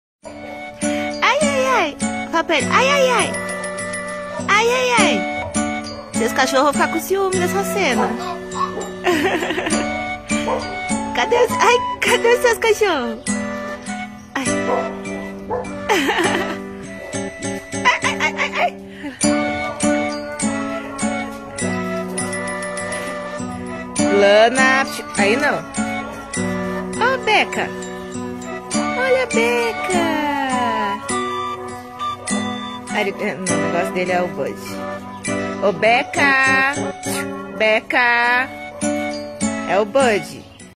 O amor aos animais está no sangue...
Matheus convive com os animais desde a barriga e  o amor parece que aumenta conforme os anos passam...